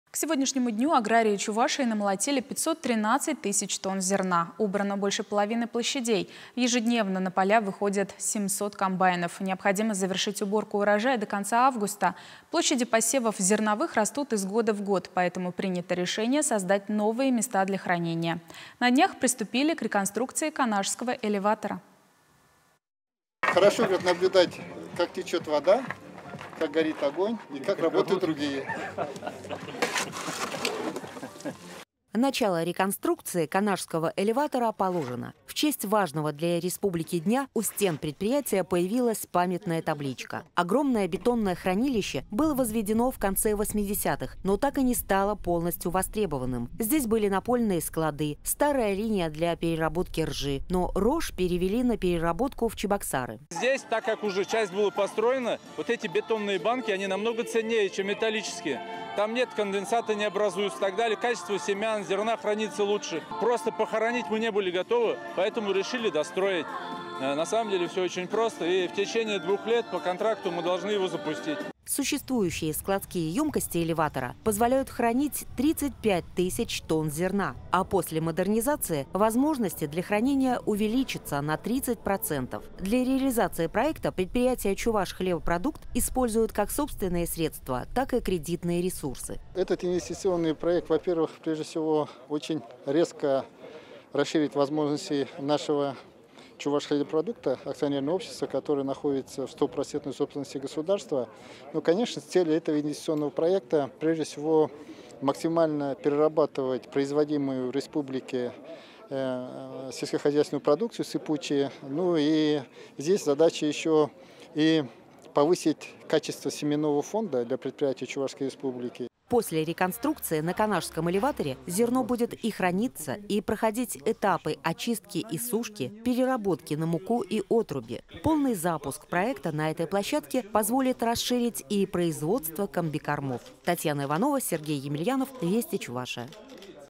В Чувашии приступили к реконструкции Канашского элеватора
В сегодняшнему дню аграрии Чувашии намолотили 513 тысяч тонн зерна. Убрано больше половины площадей. Ежедневно на поля выходят 700 комбайнов, необходимо завершить уборку урожая до конца августа.